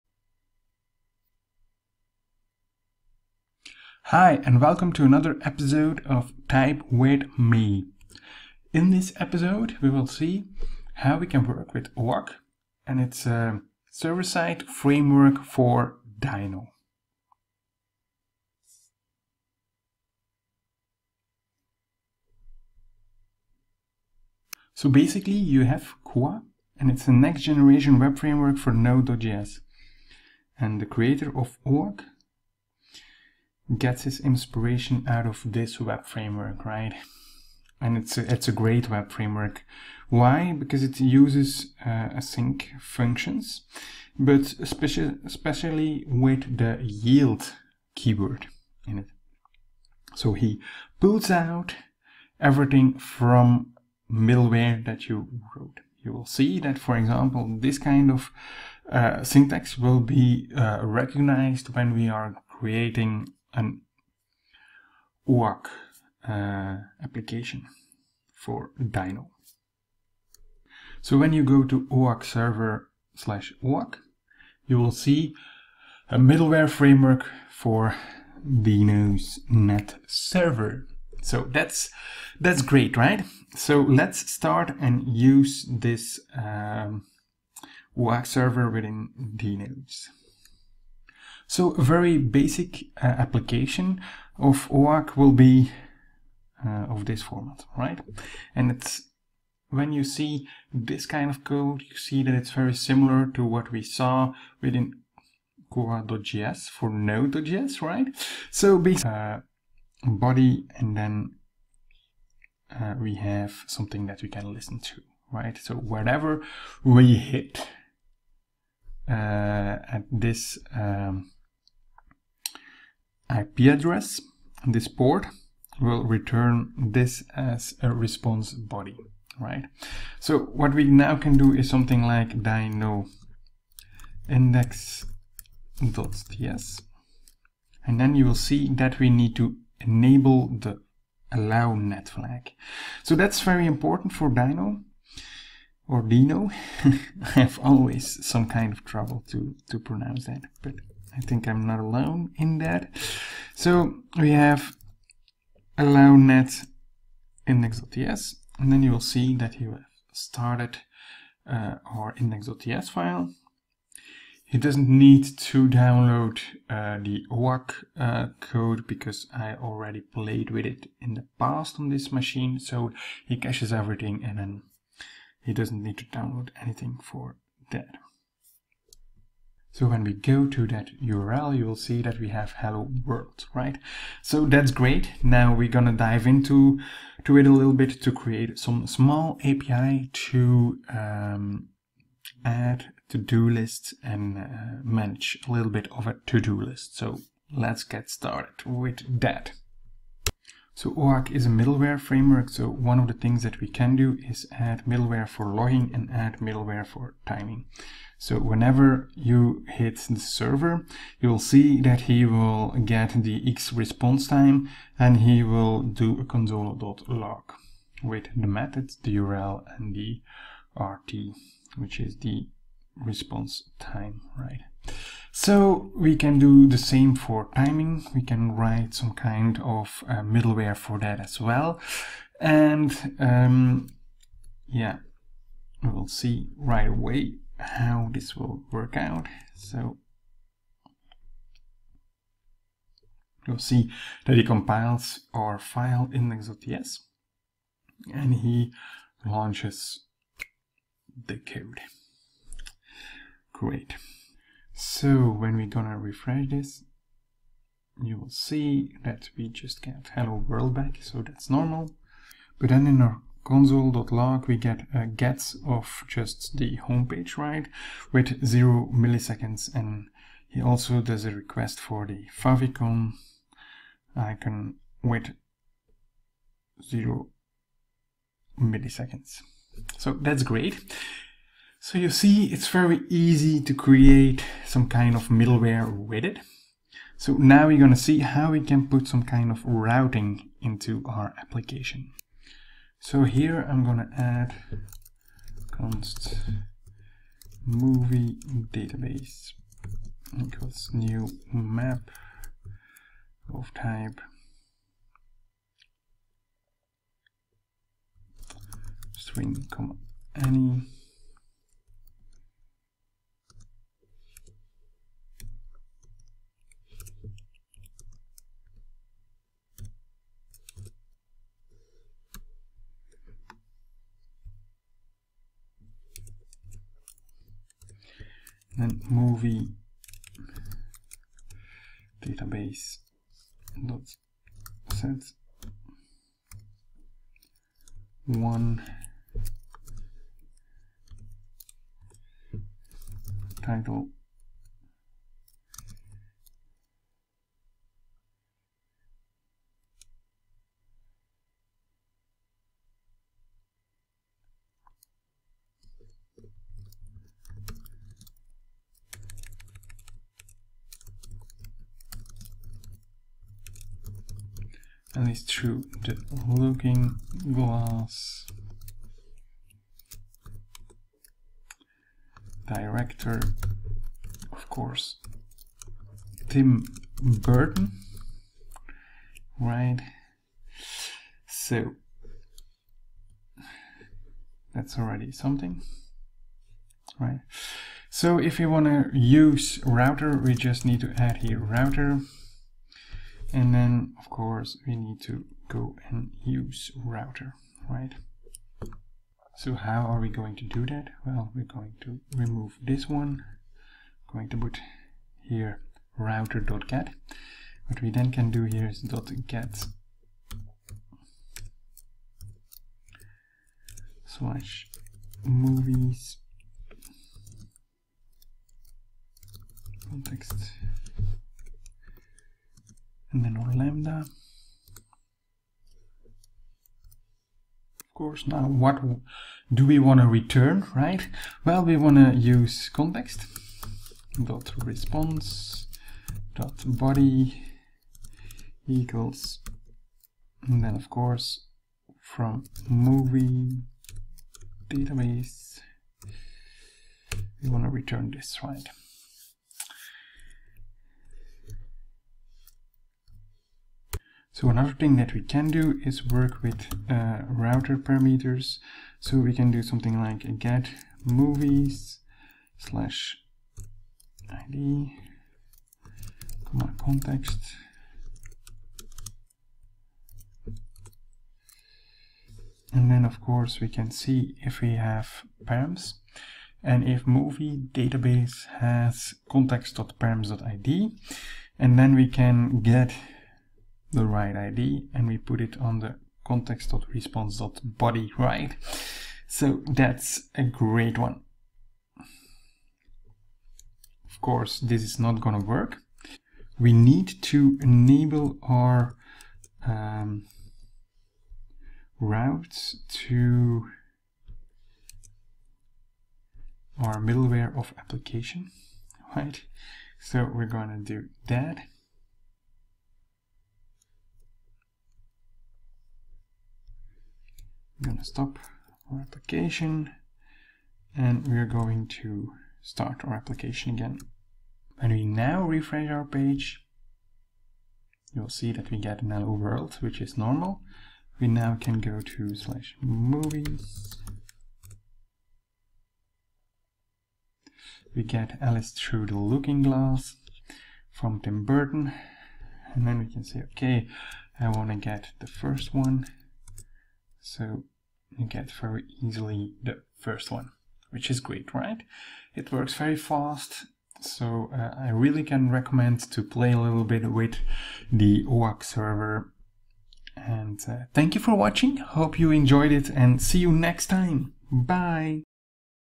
Deno oak
Deno oak, oak is a serverside framework for deno inspired by koa.js for node.js. In this tutorial we will build a basic movieDB rest api.